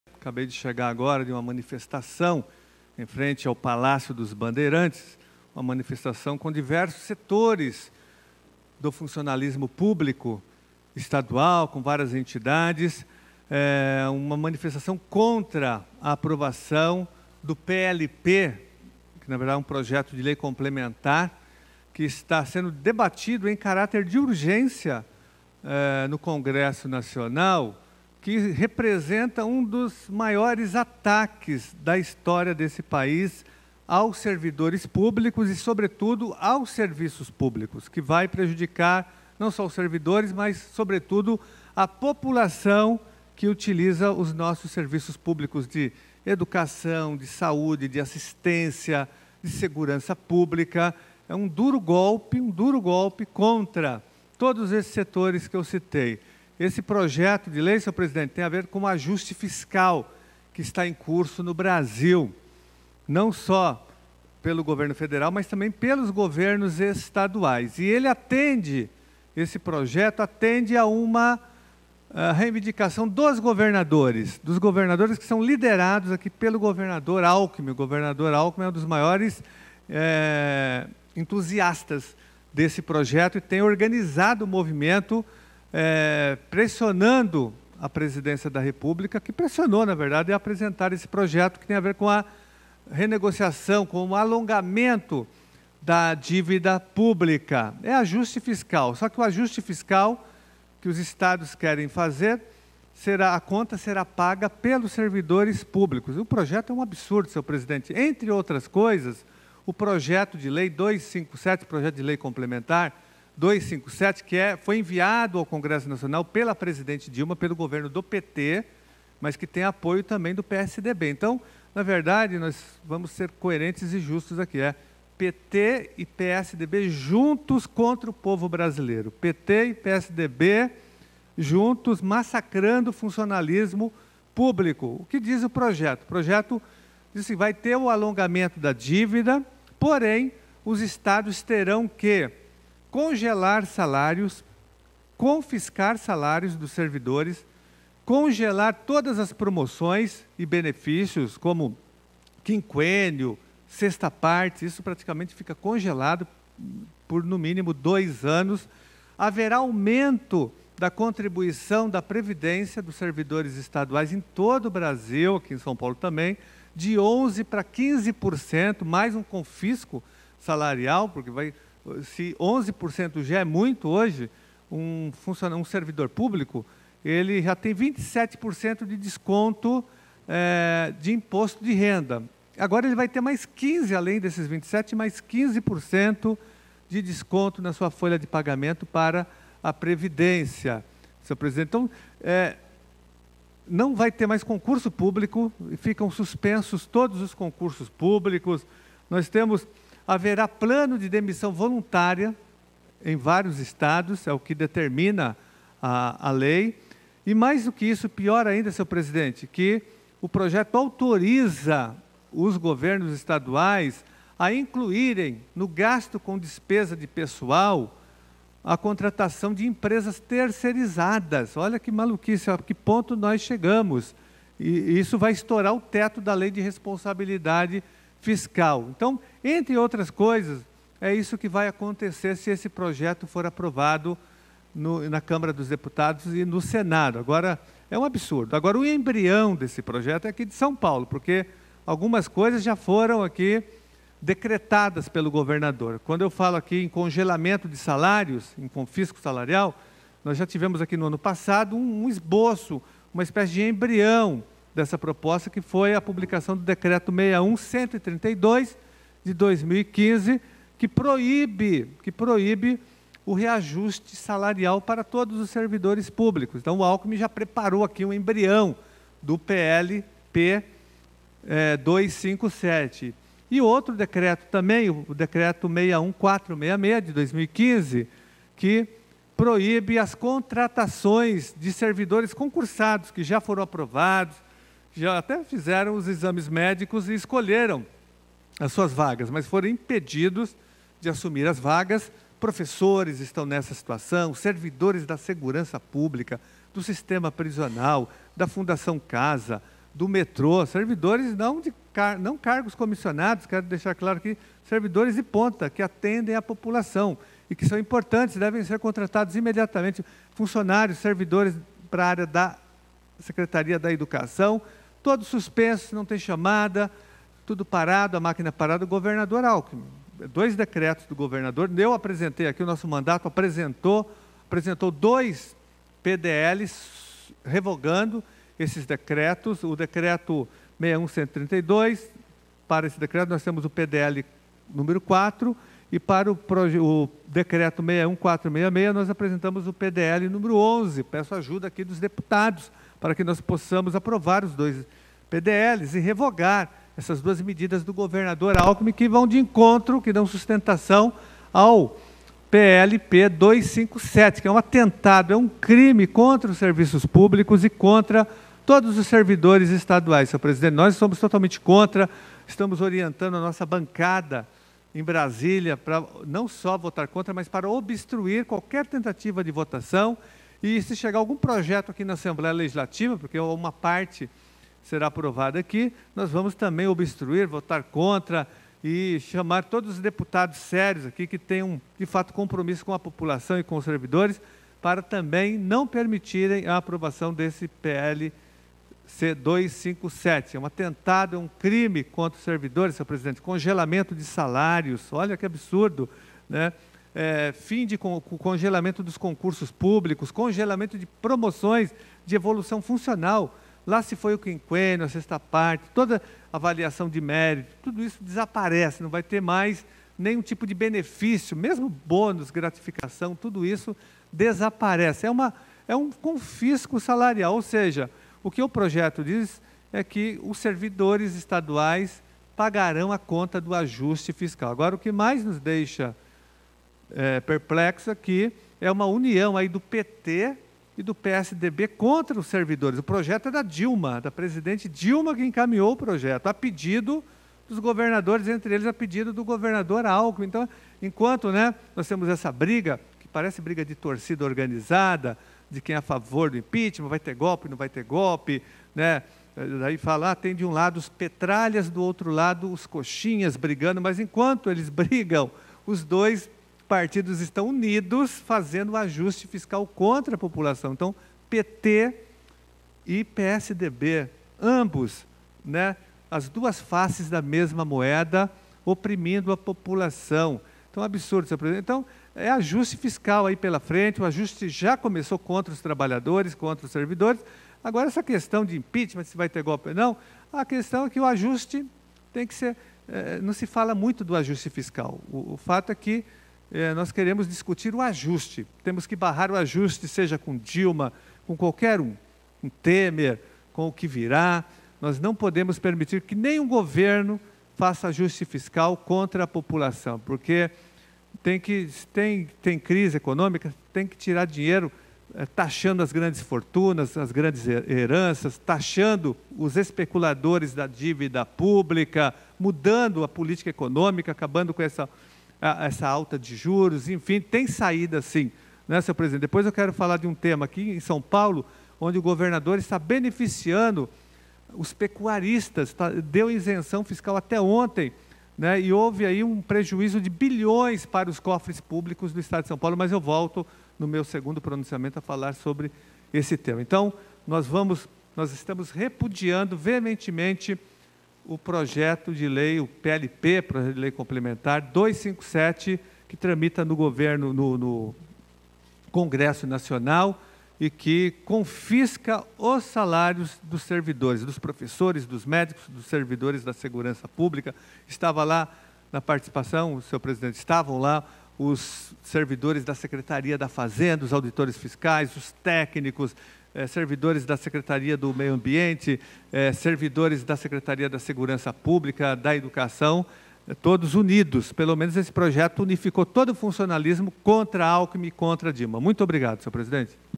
PLP 257/16 que confisca salários é criticado por Giannazi
O professor e deputado Carlos Giannazi fez críticas veementes ao PLP 257/16, de autoria da presidente Dilma, em discussão  na Câmara dos Deputados, que congela salários, aumenta a contribuição previdenciária dos servidores, congela promoções como quinquênios e sexta parte e proíbe novos concursos públicos, entre outras atitudes contra os servidores estaduais de todo o país e sobretudo contra a população usuária de serviços públicos como Educação, Saúde e Segurança.